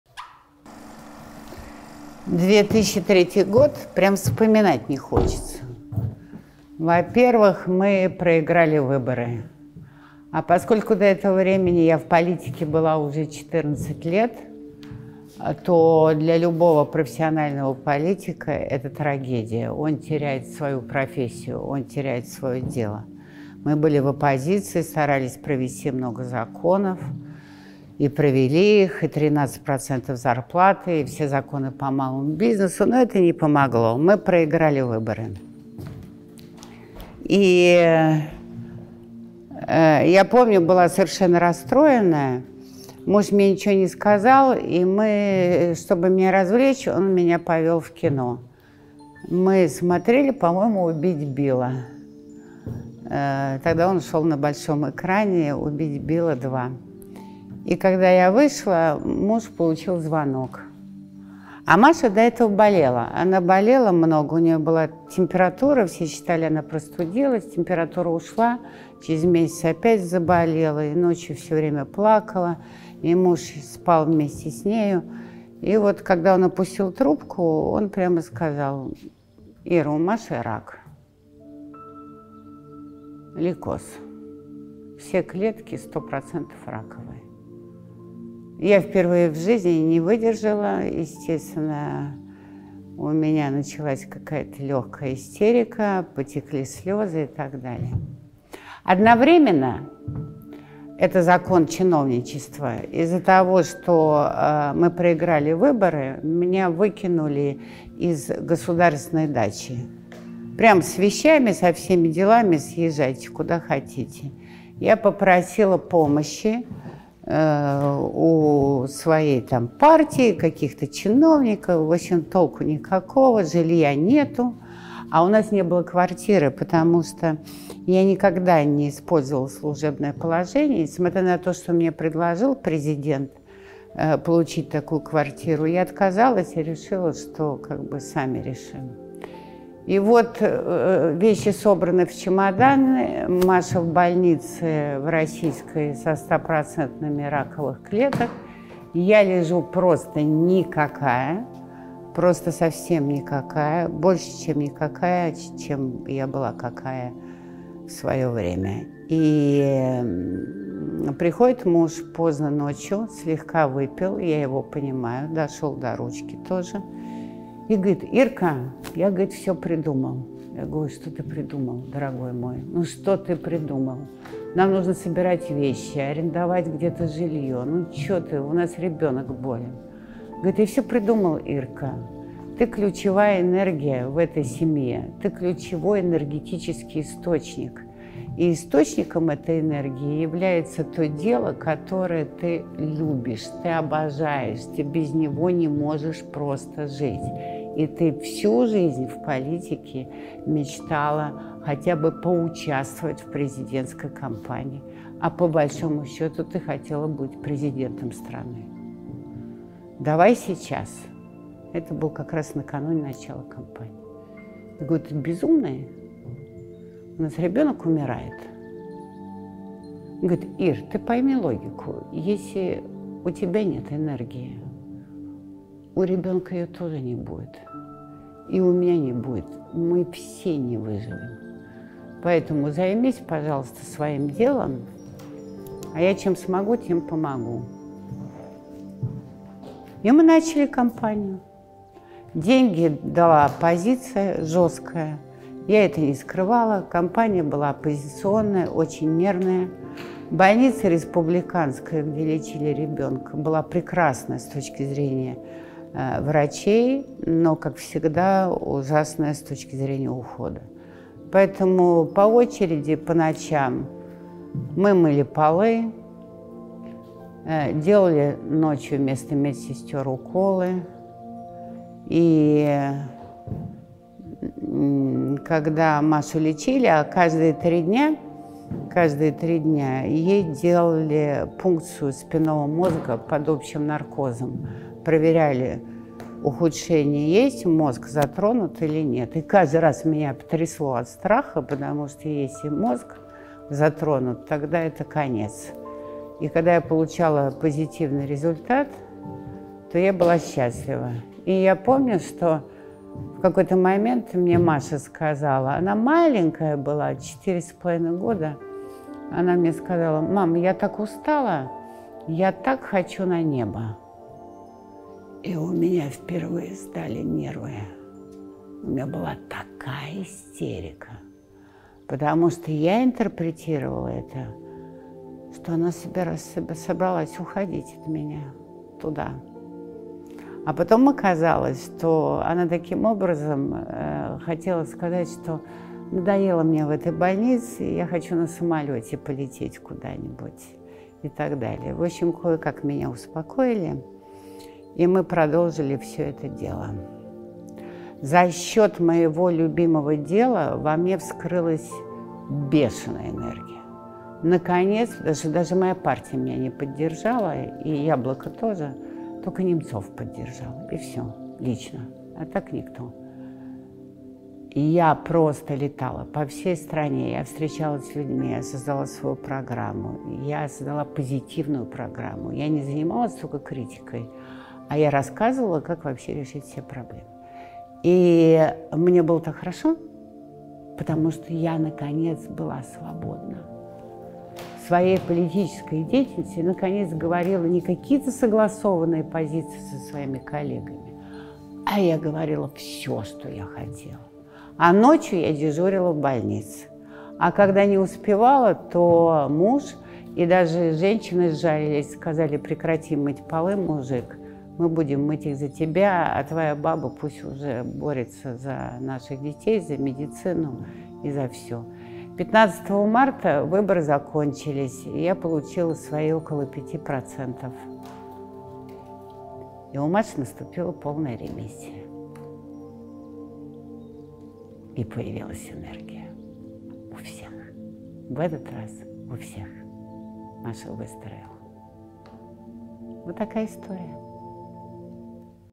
Рак у дочери, президентская кампания и все это в один год
2003 год вспоминать не хочется. Проиграли выборы, выселяют из жилья, у моей дочери рак и тут мой муж говорит: «Иди в президентскую кампанию». «Зачем» - спрашиваю я? Смотрите видео, там есть ответ.

Соберите все пазлы моего опыта и создайте свой собственный на трансформационных марафонах ХАКАМАТОН по ссылке